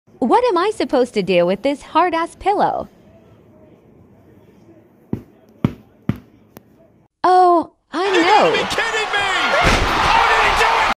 Hotel Pillow Violence
Had this hard ass pillow at my recent hotel